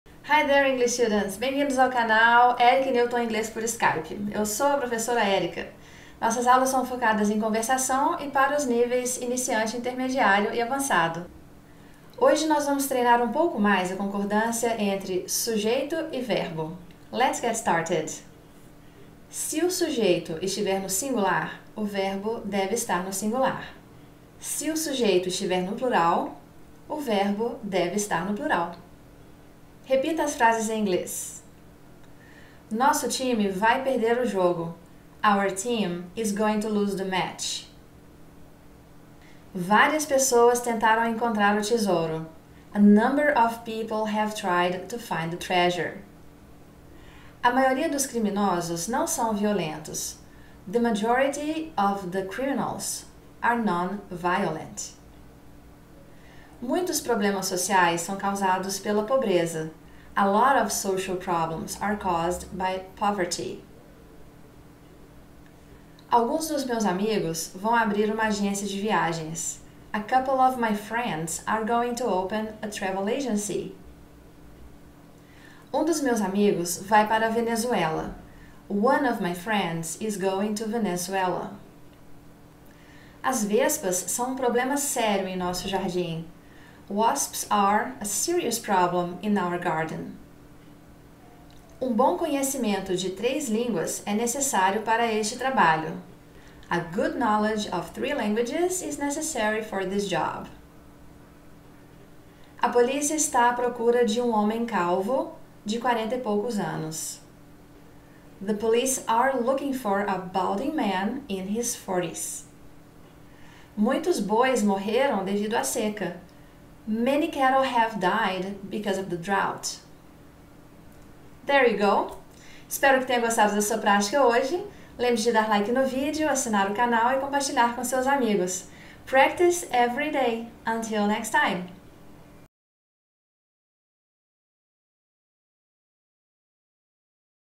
GRAMMAR: Como fazer a CONCORDÂNCIA ENTRE SUJEITO E VERBO!!! | Profa. Érika de Pádua
Tema da VIDEO AULA de Inglês de hoje:
Como fazer a CONCORDÂNCIA ENTRE SUJEITO E VERBO!!!